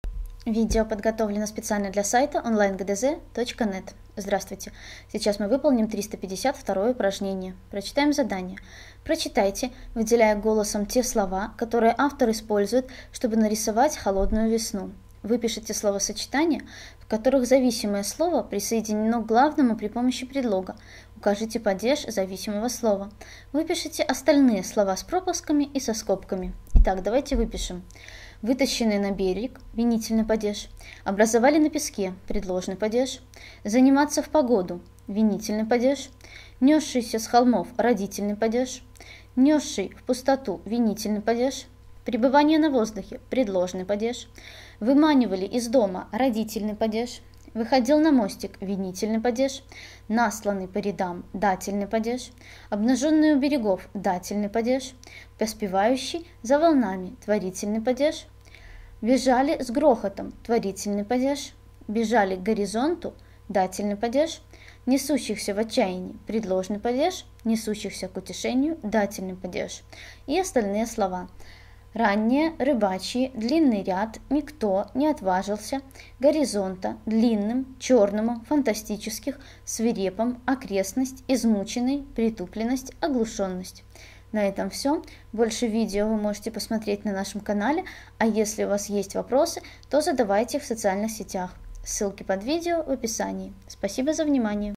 Задание № 352 — Русский язык 7 класс (Ладыженская, Баранов, Тростенцова)
Для решения 352 упражнения надо прочитать, выделяя голосом те слова, которые автор использует, чтобы нарисовать холодную весну, выписать словосочетания, в которых зависимое слово присоединено к главному при помощи предлога, указать его падеж. Это задание окажется несложными для вас после просмотра нашего подробного видео с объяснением!